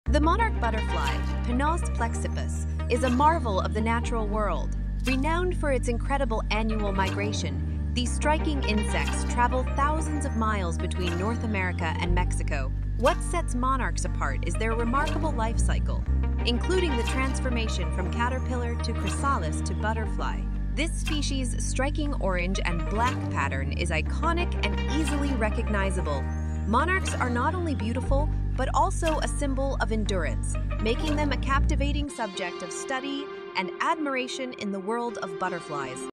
The monarch butterfly: Flowing in perfect harmony
About video
Join us on a journey into the fascinating world of Monarch Butterflies (Danaus plexippus). Witness their incredible migration from North America to Mexico, marvel at their stunning transformation from caterpillar to butterfly, and discover the vibrant beauty of their iconic orange and black wings. Learn about conservation efforts to protect these magnificent creatures and be inspired by the magic of nature.